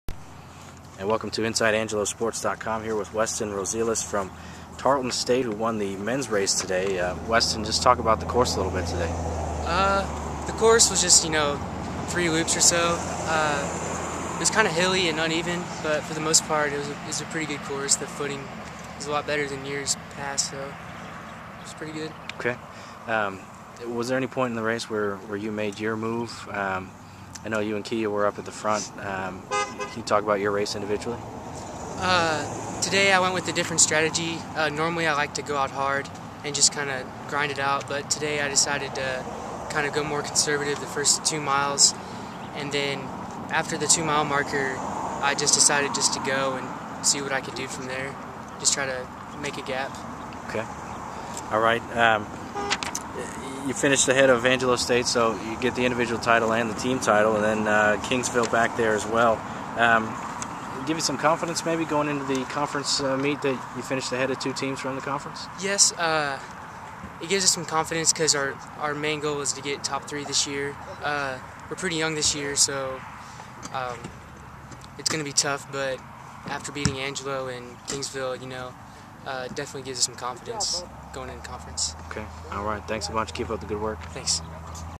Inside AngeloSports.com: ASU Blue and Gold Classic - Westyn Rosiles (TSU) (10/26/10)
Post-race comments from Tarleton State's Westyn Rosiles, the top finisher on the men's side at the 2010 ASU Blue and Gold Classic at Mathis Field in San Angelo Tuesday evening.